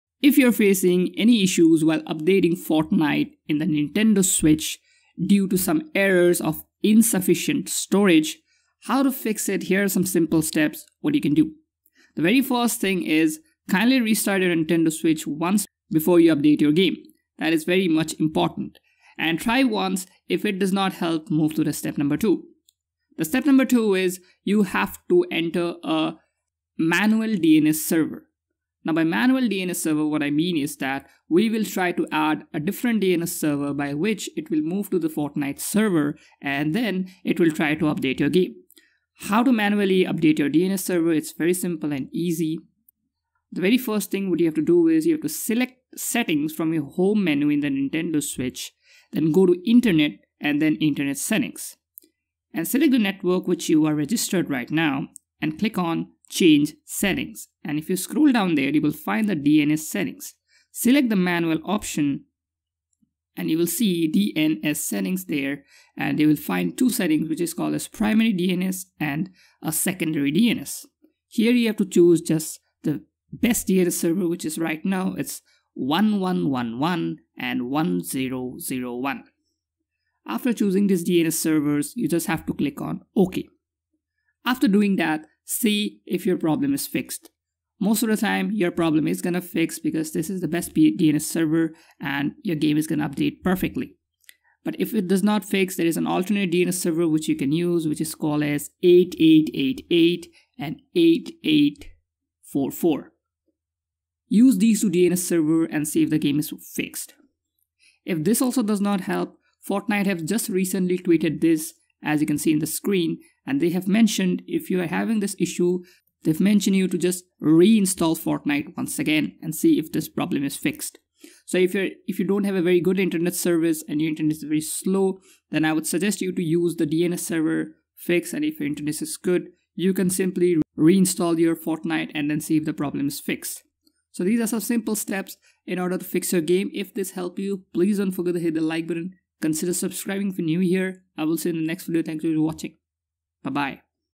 Fix : Fortnite Chapter 3 *NOT UPDATING* on Nintendo Switch Due to Insufficient Storage Space
I am going to show you how to Fix Nintendo Switch Not updating problem in Fortnite Chapter 3. If yourfacing this issue in Nintendo Switch as it is not updating to the latest Fortnite Chapter due to Insufficient Storage Space, here are some steps in order to fix your fortnite game in the NintendoSwitch.
The New Update for Fortnite Chapter 3 is here and many Nintendo Players are facing some issues  regarding the Update of the game as they are not able to Update the game to Fortnite Chapter 3 in Nintendo Switch